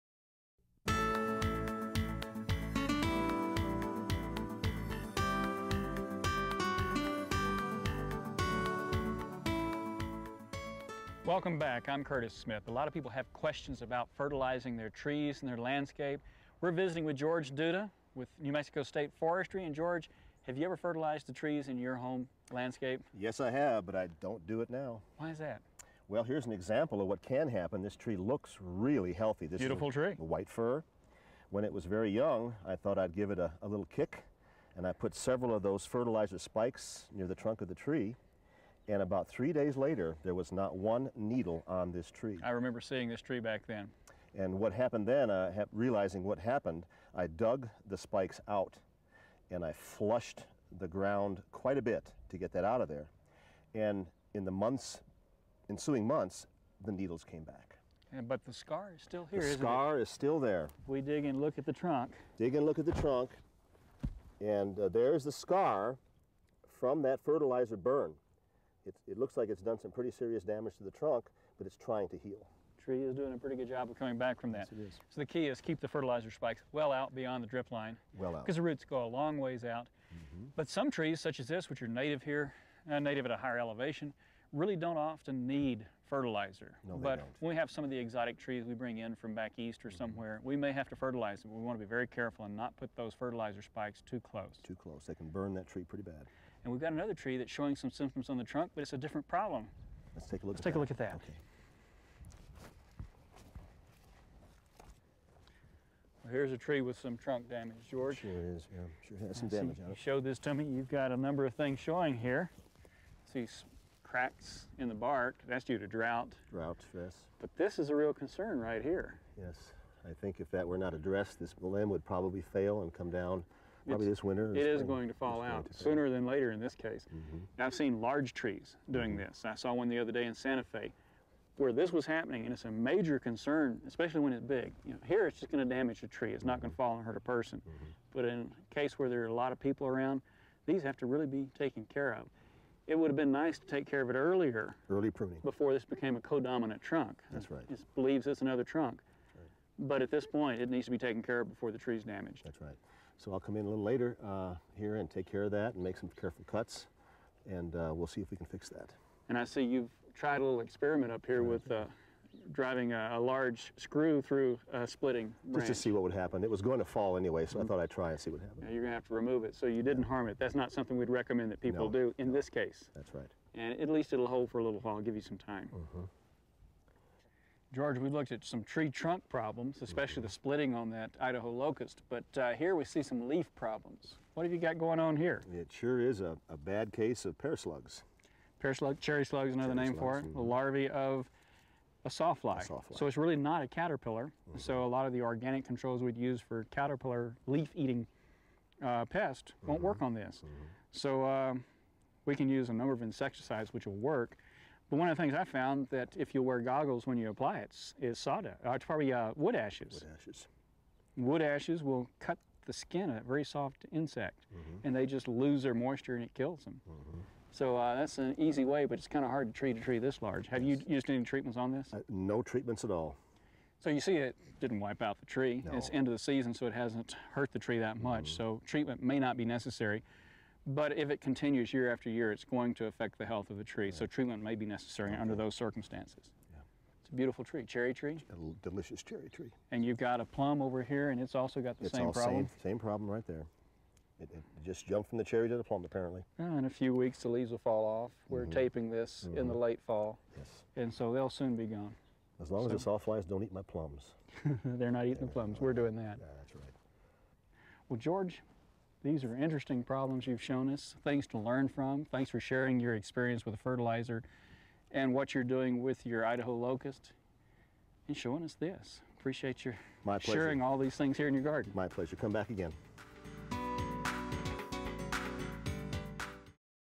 How To Identify Common Tree Problems - Over Fertilization, etc.
Learn how to identify and correct common tree problems such as using too much fertilizer, branch splitting and damage caused by the Pear Slug.